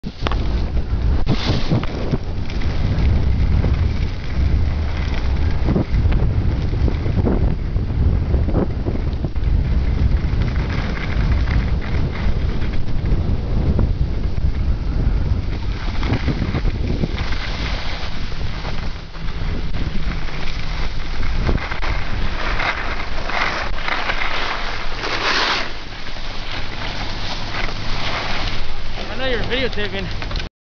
Snowshoe, WV: Day Three - Going down the mountain 2008-12-26
Going really fast down the slope!